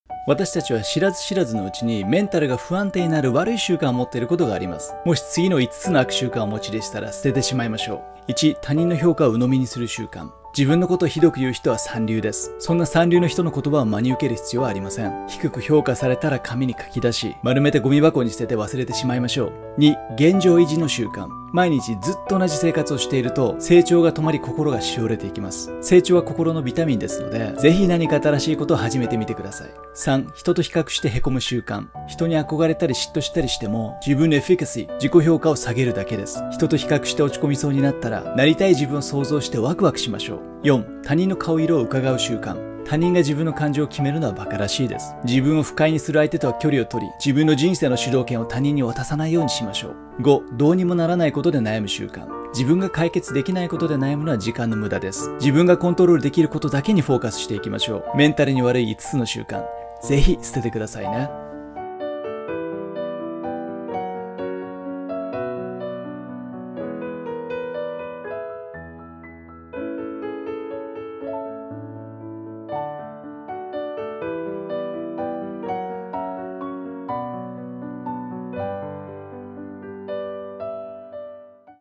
【1分メンタル】～今すぐ止めたい悪習慣ワースト５～
私たちは、知らず知らずのうちにメンタルが不安定になる悪い習慣を持っていることがあります。もし、この動画で紹介している5つの悪習慣をお持ちでしたら、捨ててしまいましょう。